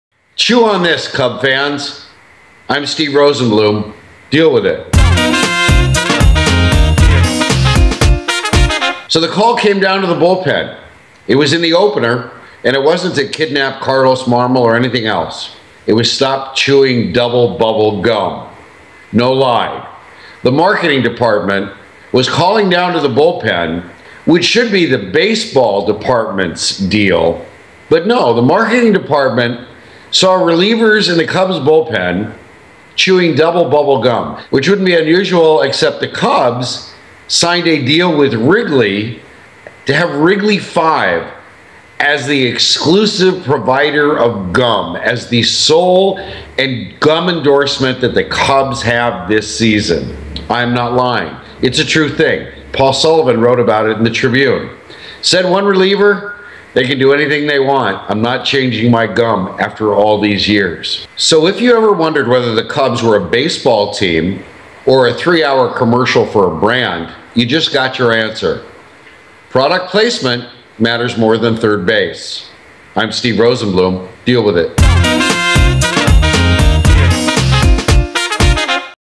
At Wrigley, product placement matters more than third base
Chicago Tribune sports columnist Steve Rosenbloom weighs in on Monday game's message to relievers to stop chewing Double Bubble chewing gum in the bullpen. (Posted on April 10, 2013.) subscribe to this channel, or follow us @TribVideo.